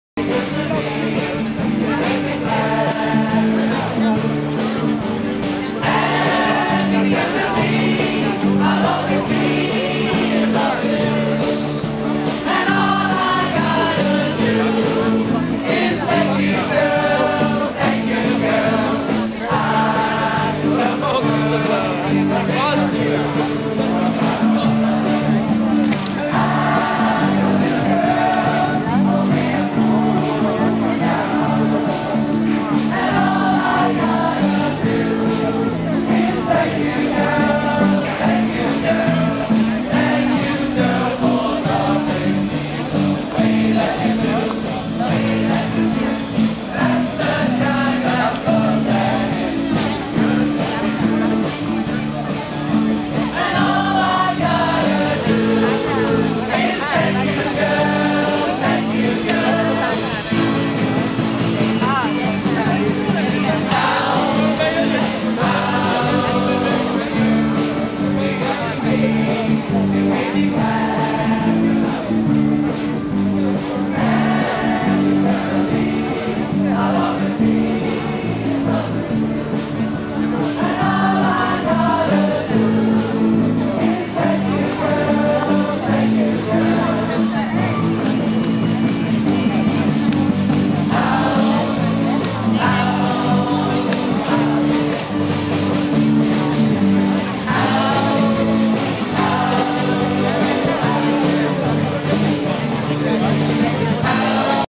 tributo ai Beatles
Questo è Strawberry Fields Park in Central Park.Il parco voluto dalla moglie in memoria di John Lennon qui assassinato